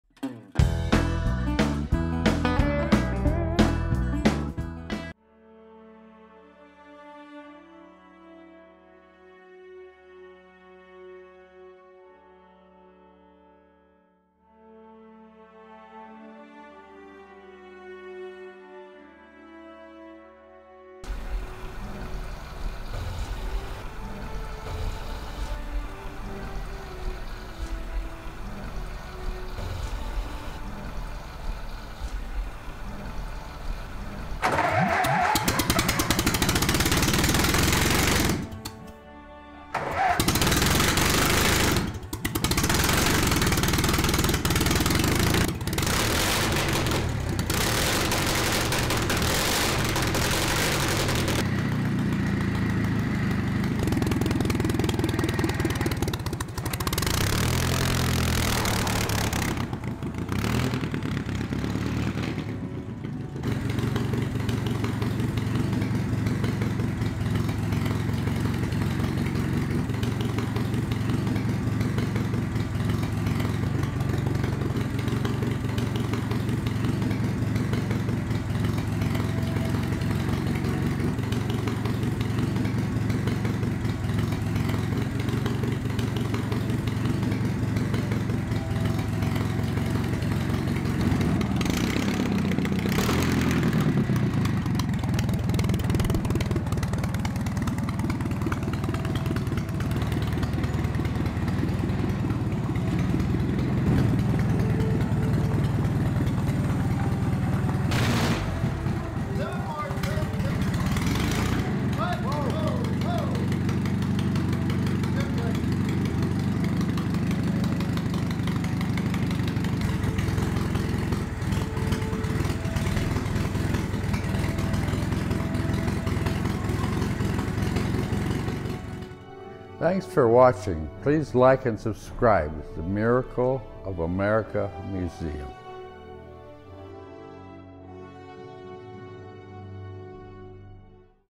Model T Ford Speedster
In this exciting video, Gil takes the 1926 Model T Ford speedster out for a drive around the Miracle of America Museum. This vehicle is a period correct build and restoration by Gil himself. It’s also equipped with period correct speed equipment as well.